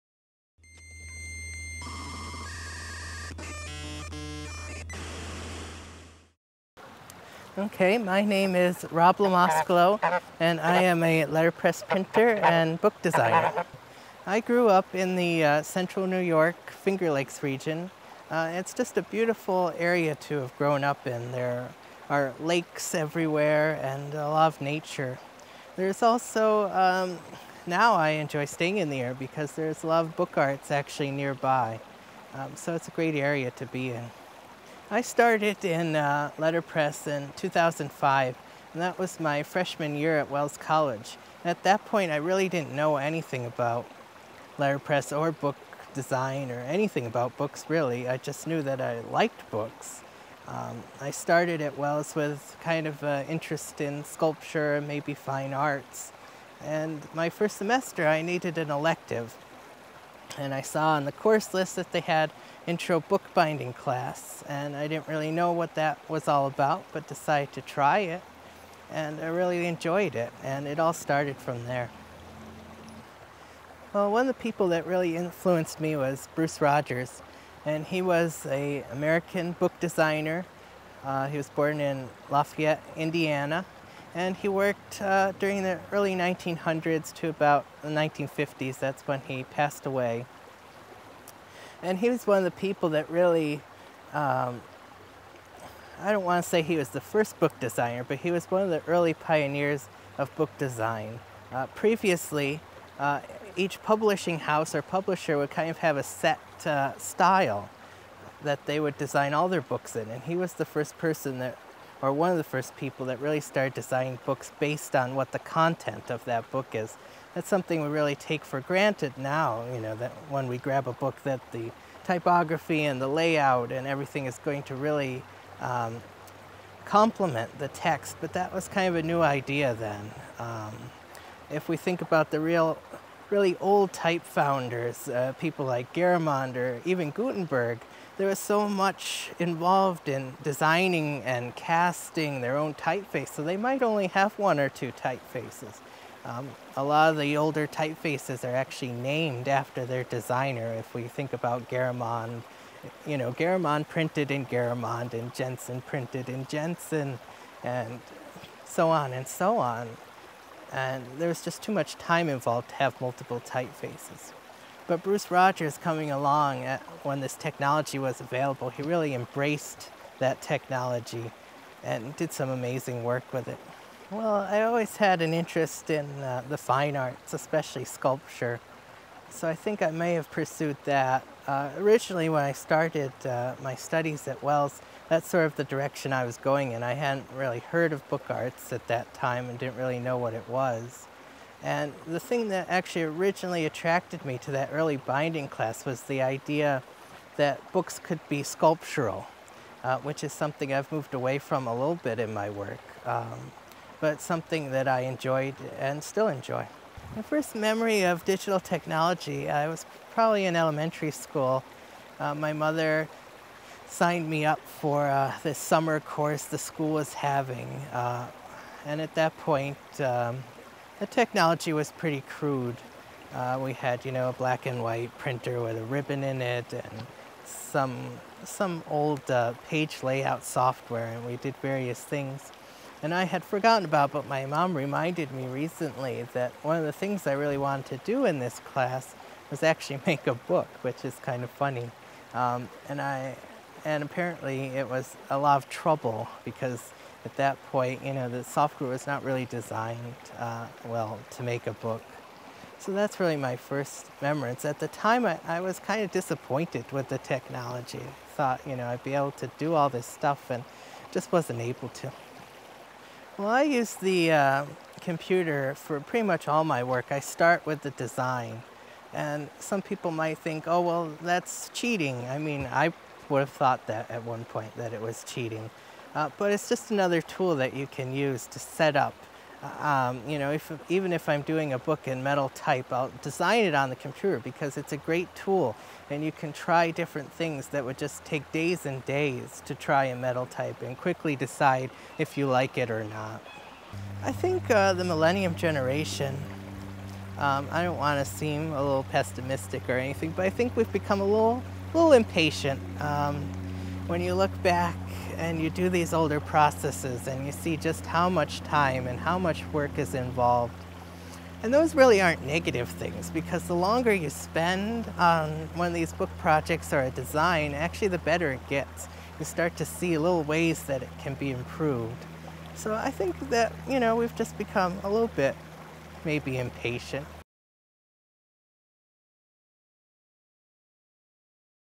Rob: Bio
Rob LoMascolo began studying letterpress and book arts at Wells College, in Aurora, NY. There he studied with Terry Chouinard, Herbert Johnson, Mark Argetsinger, and Michael Bixler. He received an MFA from the University of Alabama and apprenticed under Michael and Winifred Bixler at The Bixler Press & Letterfoundry. He is currently a freelance letterpress and book arts artist in Union Springs, NY. Some of his clients included The Frick Collection, members of the Grolier Club, Wells College, private book collector, and conservators.

Fortnight Journal: Edition IV
For more on Rob and to see more of his work, visit Fortnight Journal and Rob's site at rlpress.blogspot.com